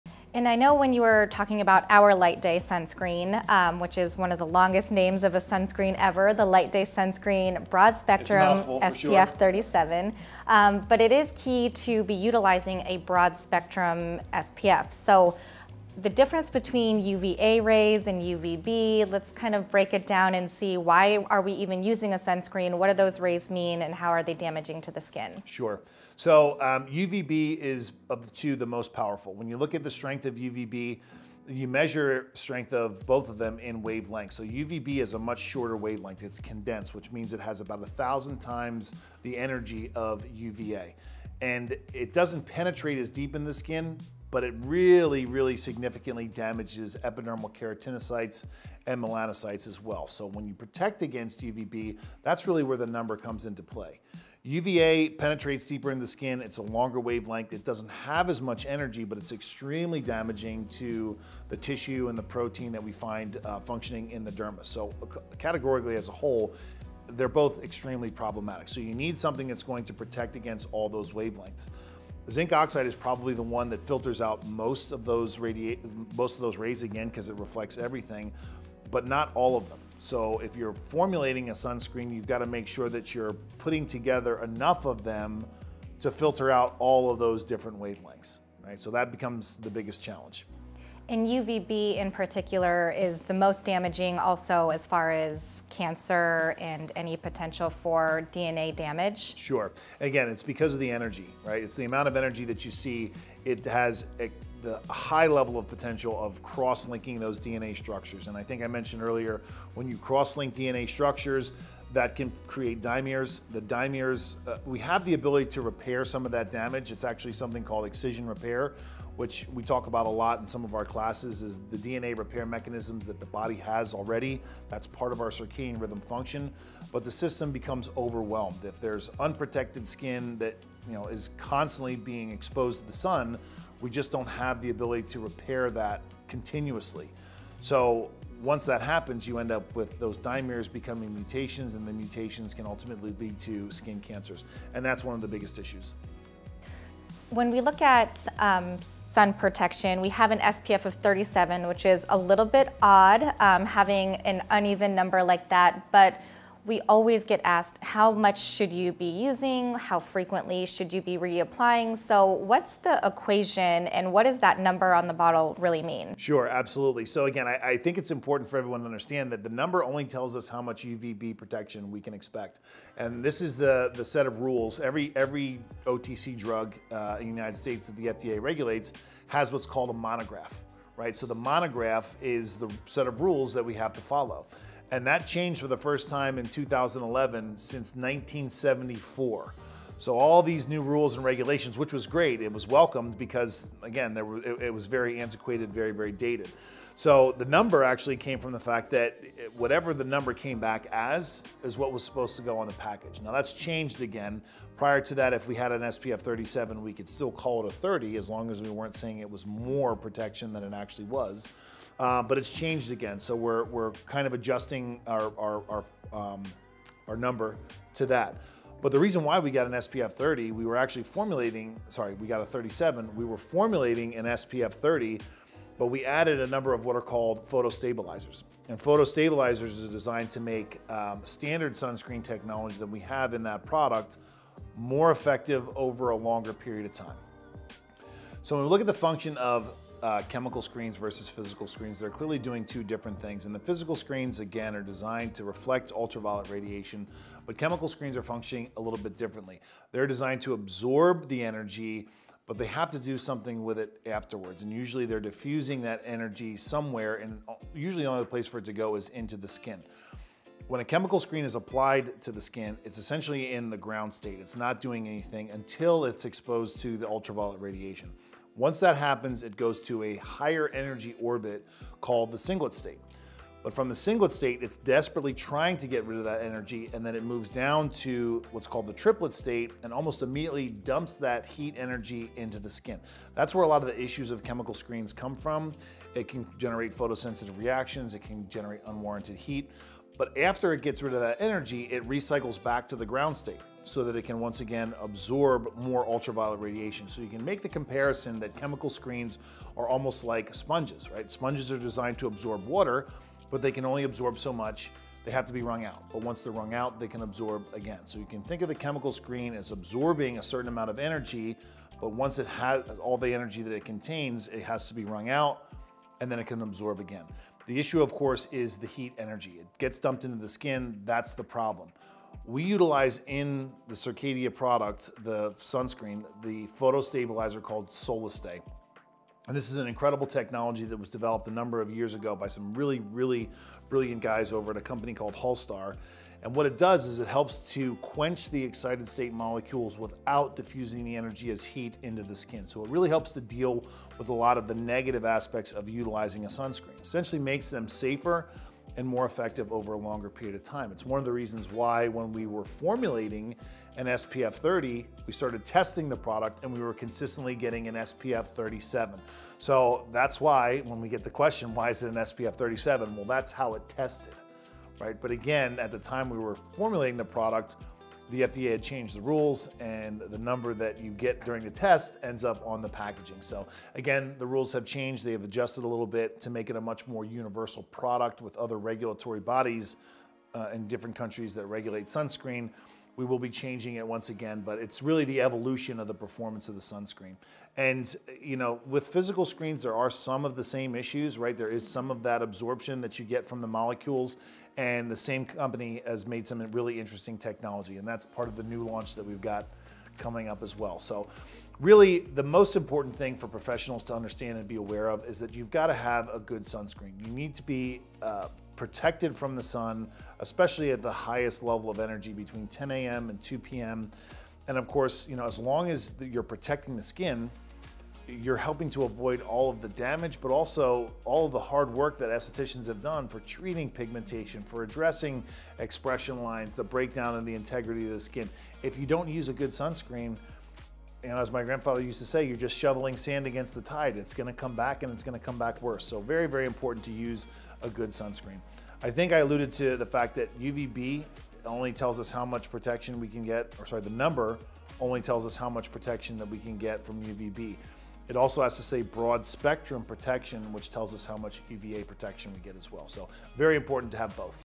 Chemical vs Physical Sunscreen + UVA & UVB Rays
A masterclass on the science and technology behind all things SPF and sunscreen. Michael Pugliese talks about how the technology of sunscreen works with UVA & UVB rays, a light history on manufacturing process on sunscreens and much more.